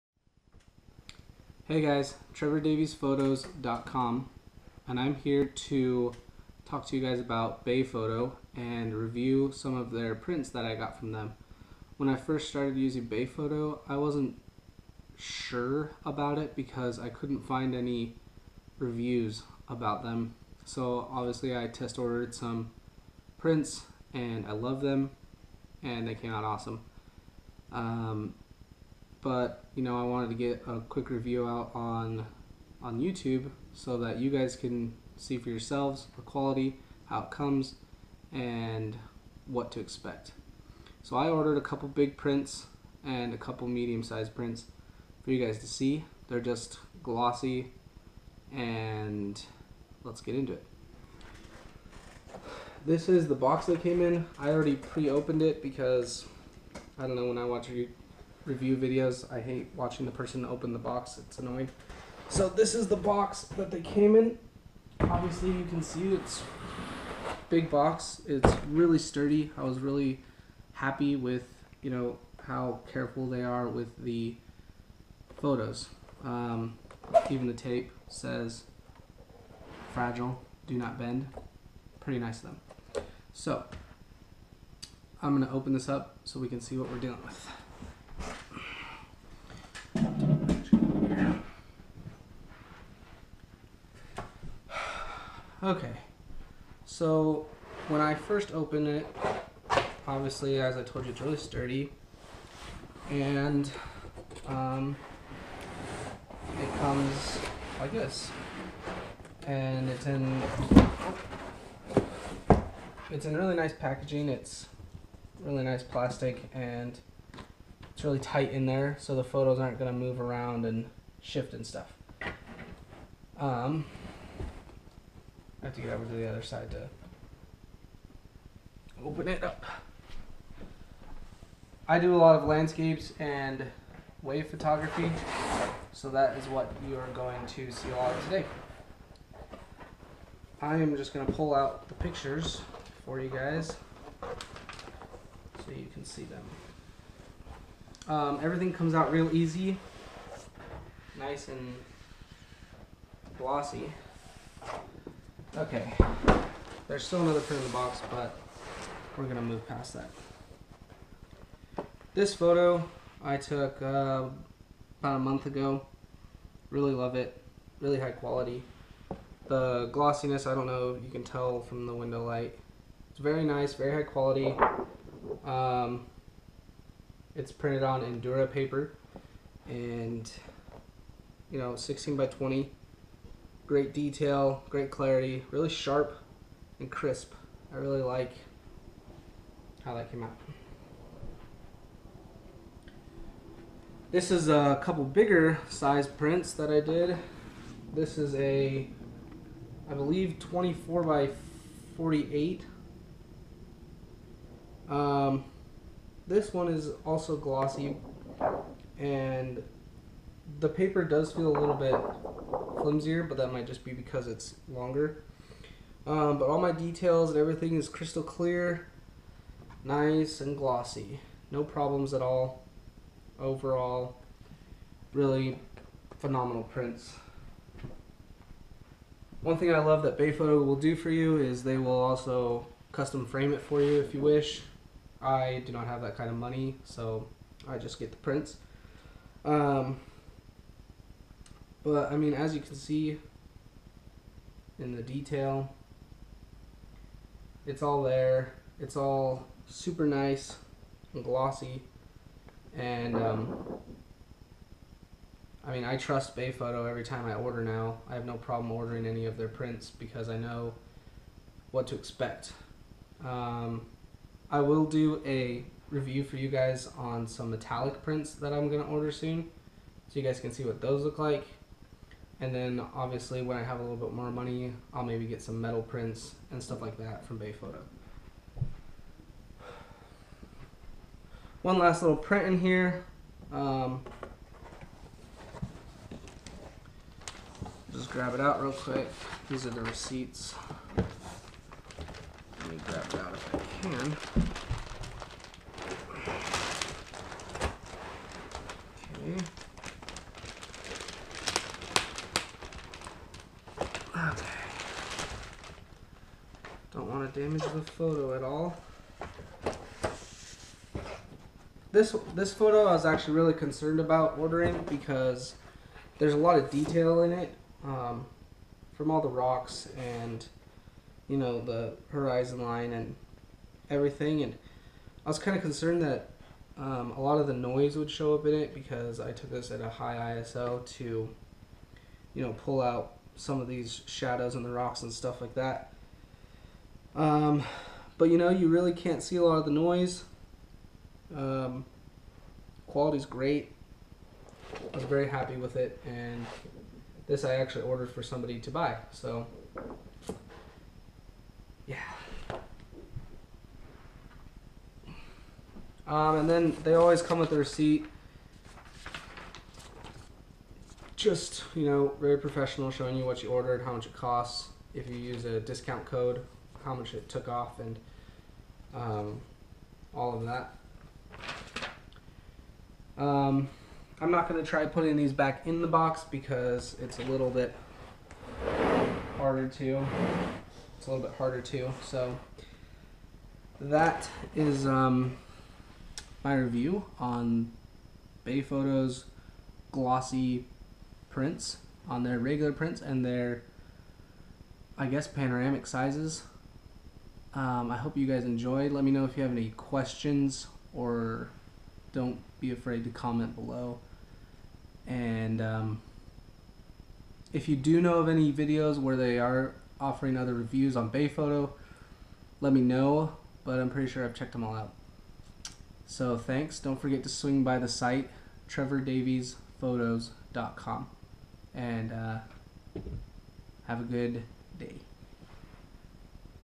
Bay photo Glossy print Review
Just a quick video I put together showing you guys what the glossy prints from bay photos look like. Sorry its not the best, I filmed this using the go pro hero 3 with no studio lighting or microphones. But you should get the point.